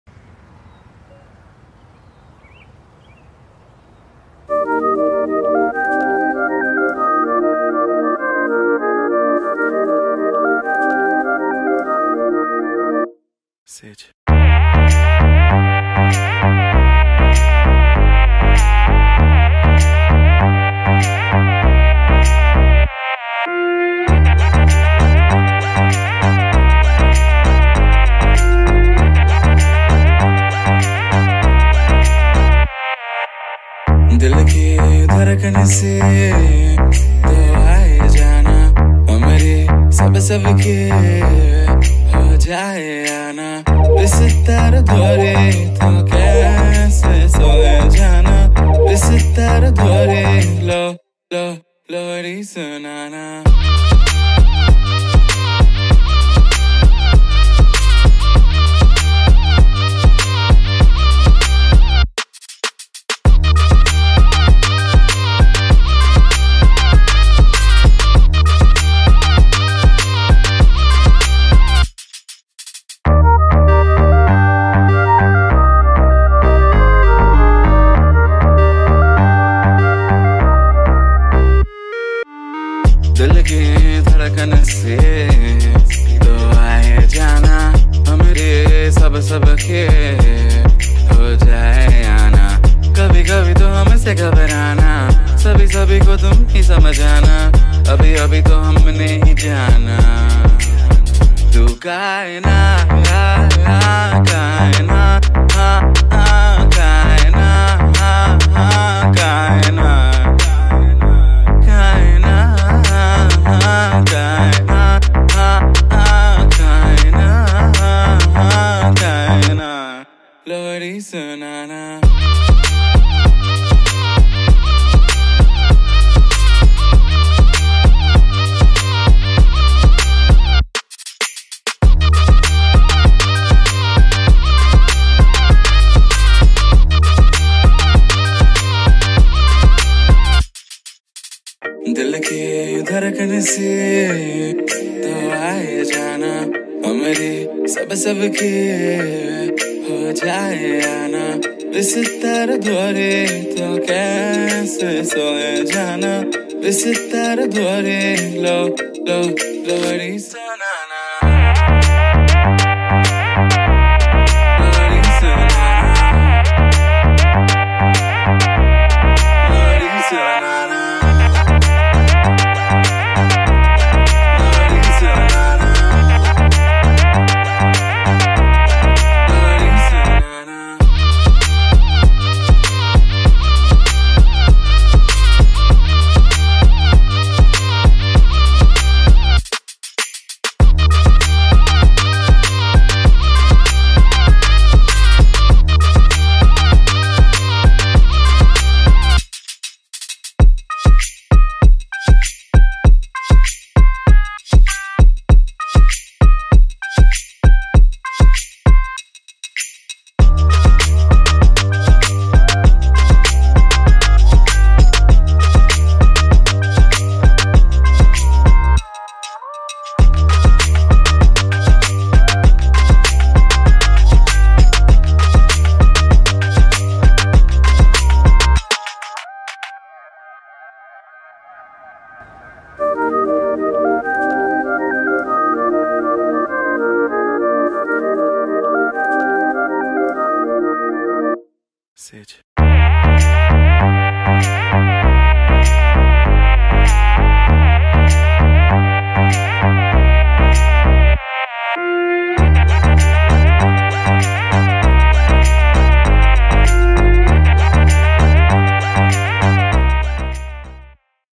Our Lock-down Diary || Team The Bong Photographers
Our Lock-down Diary, the team members who always  work behind the camera to capture and store all the sweetest memories of your best days. meet with the team members( few members left though due to some problems)

appearance and shoot done by :  Indrajit, Arka, Shuvo, Ganesh, Joy, Debashis, Anirban, Amit, Ritwik, Sovan, Pradip, Sandipan, Pratham,  Dipanjan, Sanjay, Abhi

Edited by: Sandipan